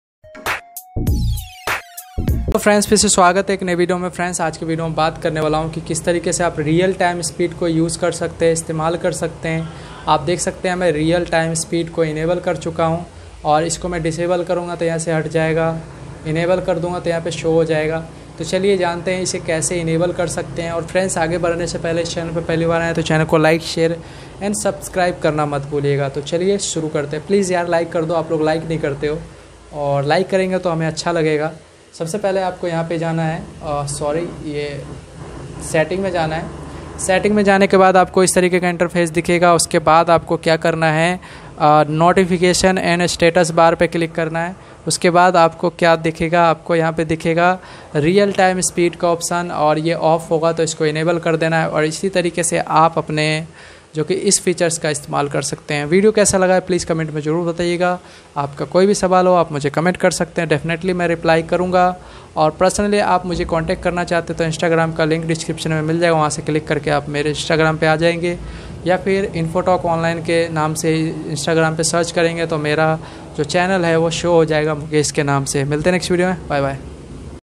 Realme C11 data speed setting | How to data connection problem | Realme speed enable kaise karen
Welcome to "infotalk online" my motive behind creating this channel is to make it easy to understand tech videos in Hindi.